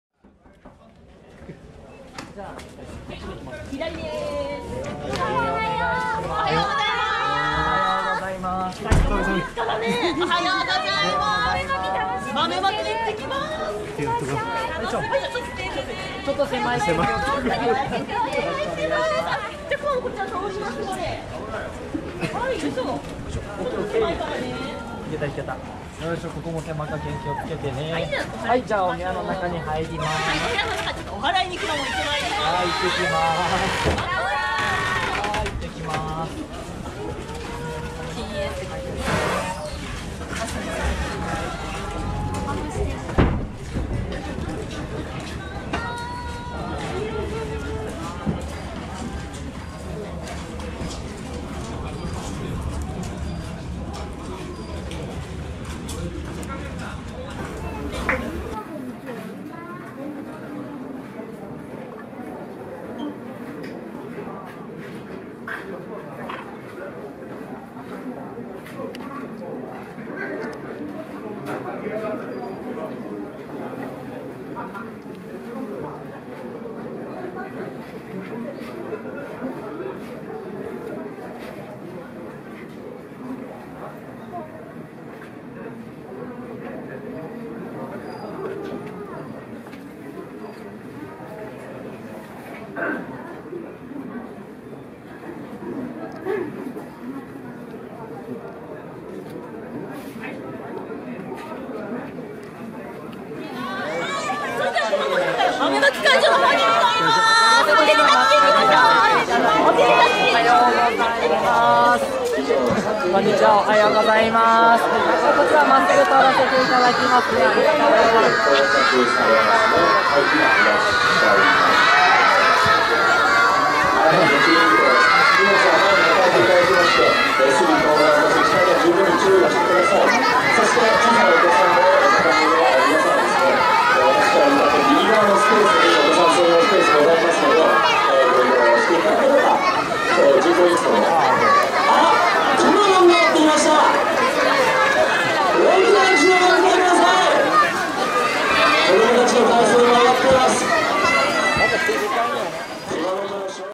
くまモン登場～お祓いをしてもらいます！＠節分大祭1回目【櫛田神社】20200203
今年もお櫛田さんの豆まきにくまモンが登場です。